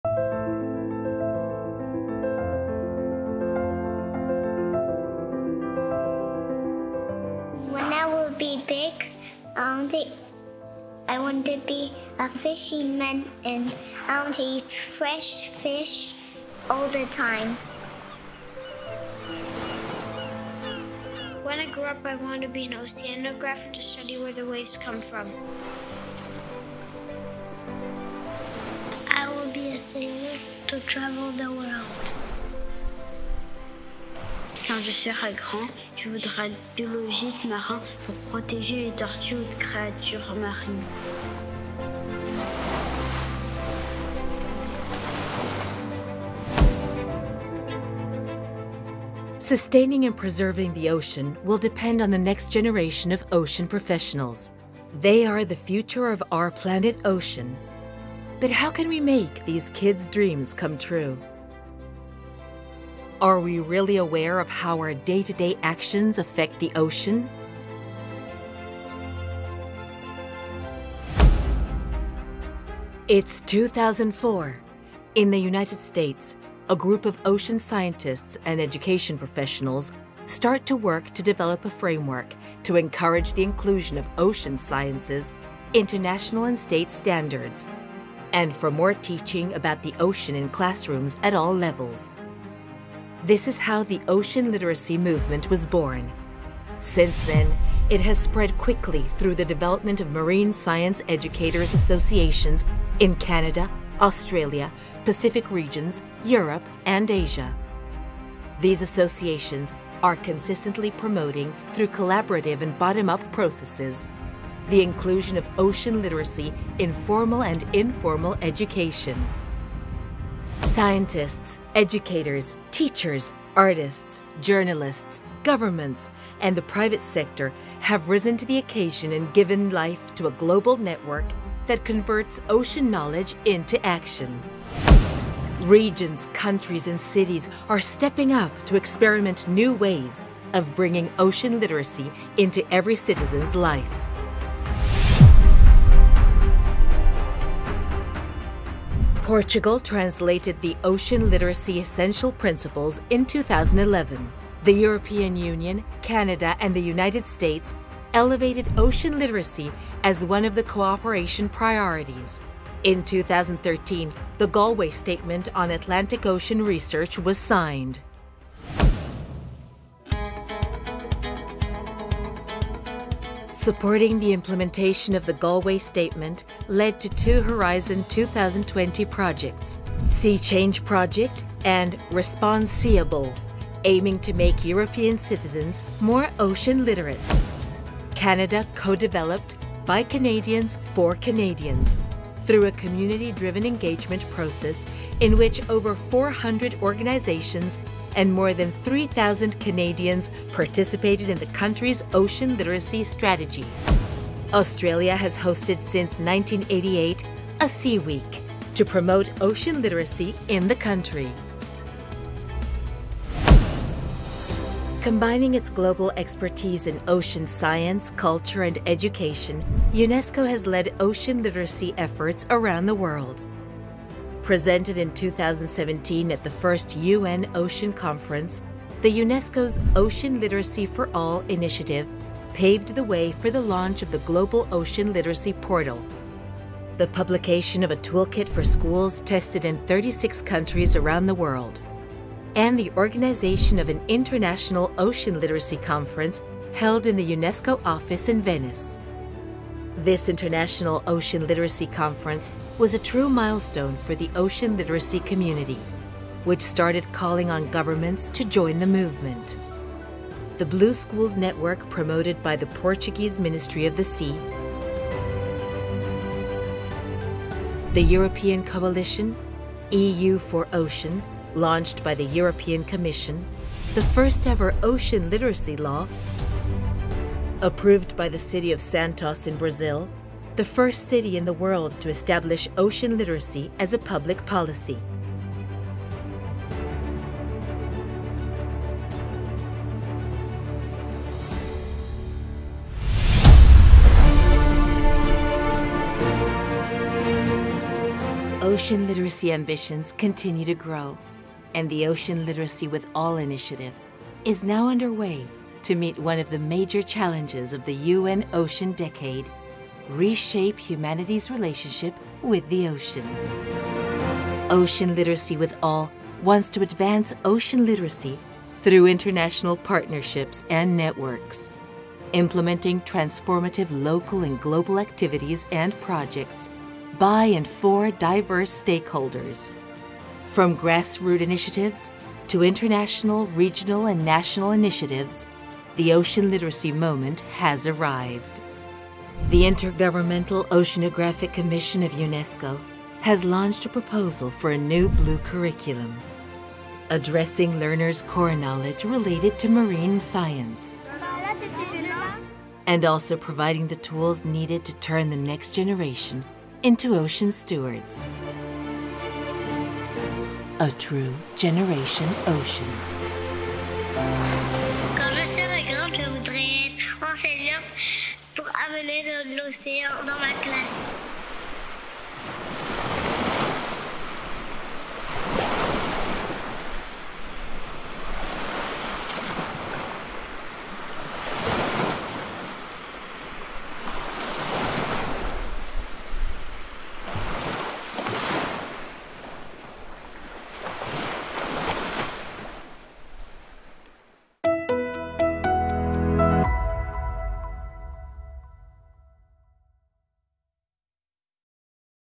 Ocean Literacy: History and Future (Full video)
The ocean literacy movement started in 2004 and since then has been able to develop numerous successful initiatives. The UN Ocean Decade will help scale them up and pave the way for the implementation of new ones, such as the proposal for a new blue curriculum.